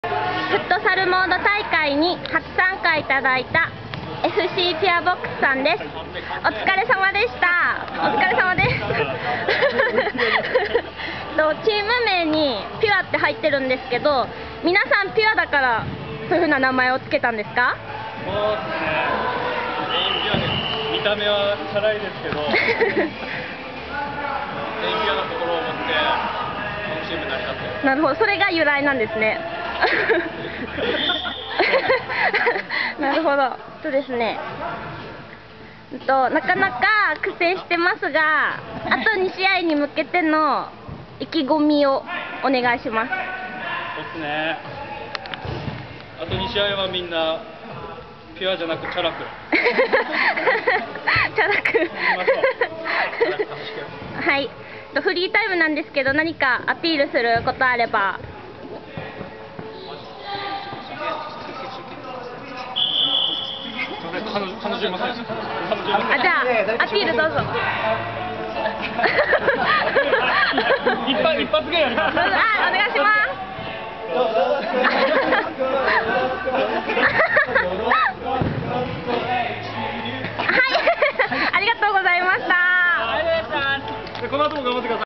2011.5.22フットサルモード主催SS-mdoeサンビレッジ紫波大会の風景.AVI
動画は2011年5月22日に開催した、フットサルモードが主催する、岩手県サンビレッジ紫波会場で行われたフットサルイベントの様子です。 この動画は、スポーツイベント＆スポーツアイテム販売のエフスポが運営するフットサルイベントの動画です!